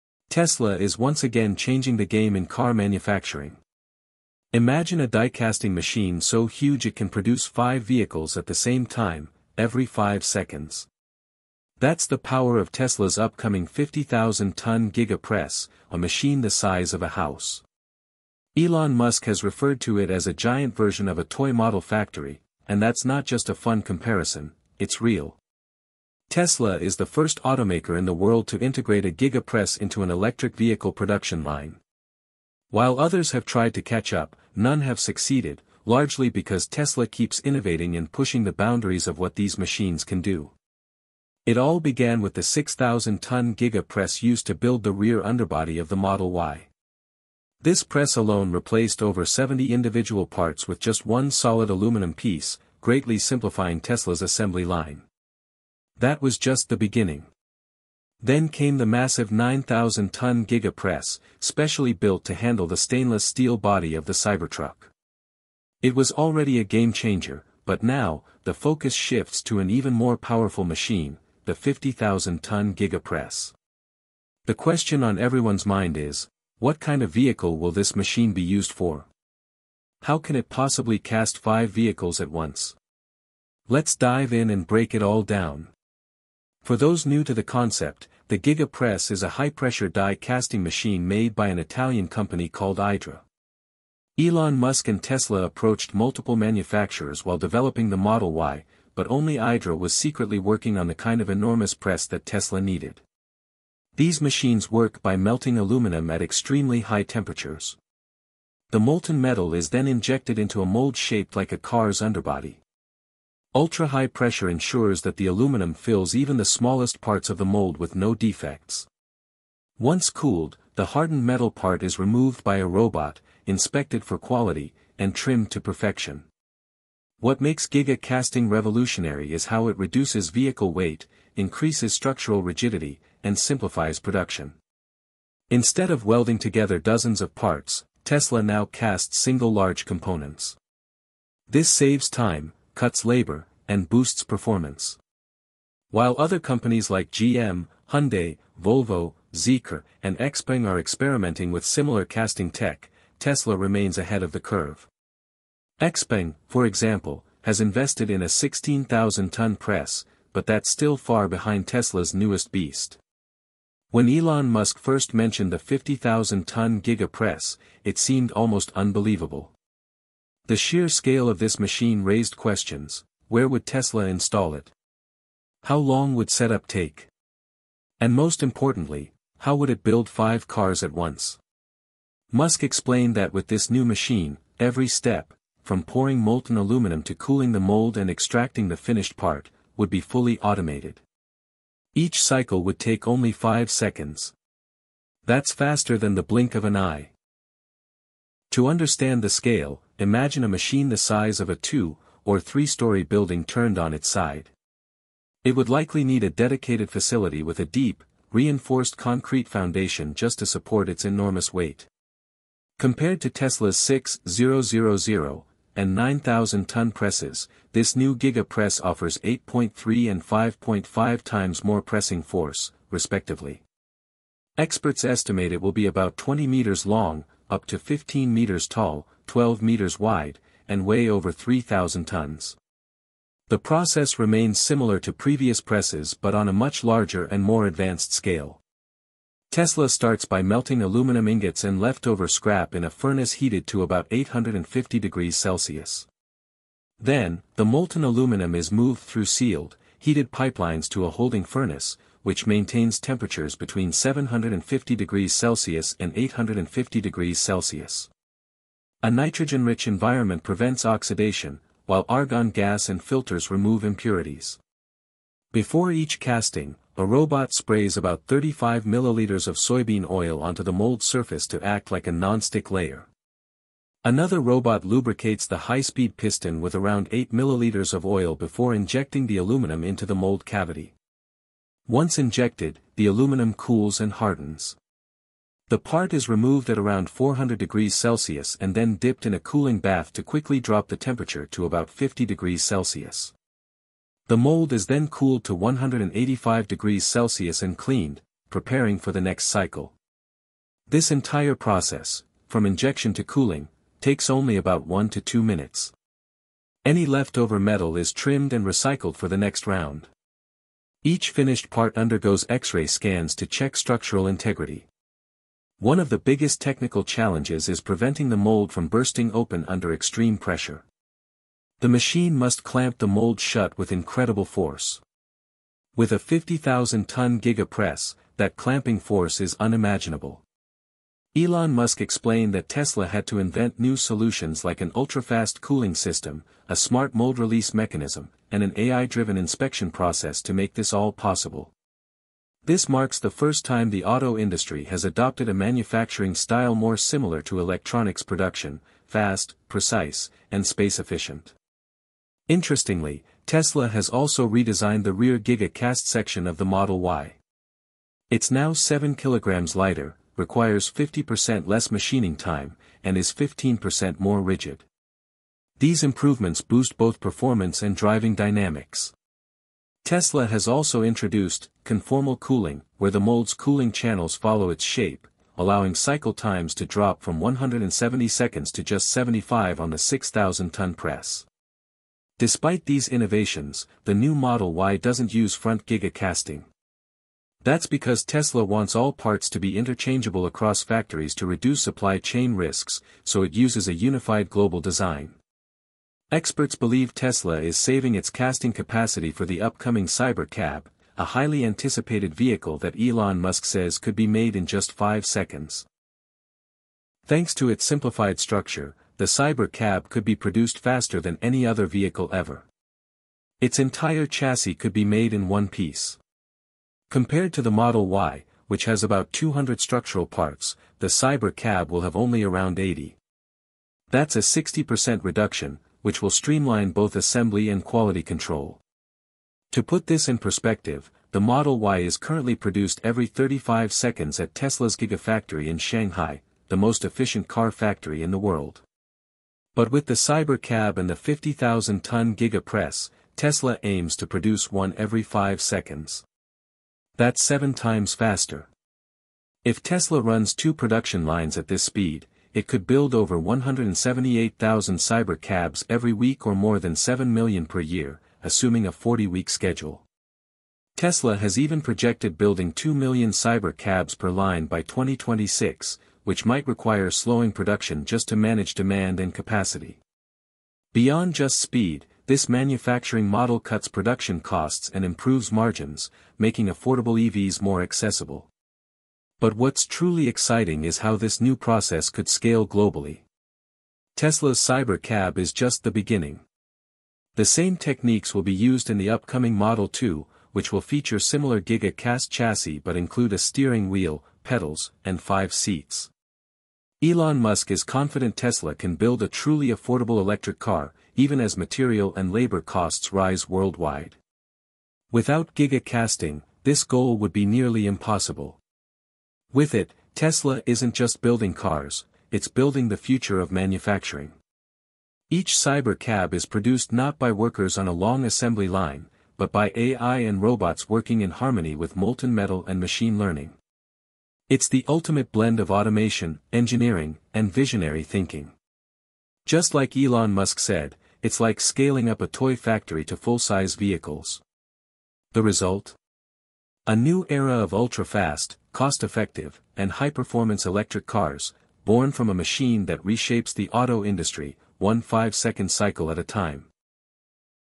It Finally Happened! Tesla’s MASSIVE 50,000-Ton Giga Press Builds a New Car in Just 5 Seconds!
HOA KỲ
It’s Finally Here! Tesla’s Mind-Blowing 50,000-Ton Giga Press Can Build a Car in Just 5 Seconds!
Imagine a giant machine the size of a house—capable of producing a brand-new Tesla every 5 seconds. Elon Musk calls it a “massive version of a toy model factory,” and he’s not joking. This incredible machine can cast up to five cars at once. It may sound unbelievable, but it’s 100% real. Tesla is the world’s first car company to fully integrate a Giga Press into its EV production process. While other manufacturers are still trying to catch up, Tesla keeps racing ahead—thanks to Elon Musk constantly upgrading the tech behind it.

Let’s take a quick trip back. It all began with Tesla’s 6,000-ton Giga Press, used to make the rear section of the Model Y. This machine replaced more than 70 separate parts with just one single aluminum piece, speeding up production like never before. Then came the 9,000-ton Giga Press—built to mold the body of the huge stainless steel Cybertruck. That machine was already a beast… but now, Tesla is taking things to an entirely new level with the upcoming 50,000-ton Giga Press. How powerful will it be? Which new Tesla vehicle is it designed for? And how can it create five vehicles at once?

Let’s dive into the details—Welcome to Car Champions.

Before we go any deeper, let’s quickly explain what the Giga Press actually is. It’s a high-pressure die-casting machine that Tesla gets from an Italian company named Idra. The story began back when Elon Musk was developing the Model Y. Tesla wanted to completely change how the car’s underbody was built—but the technology they needed didn’t exist yet.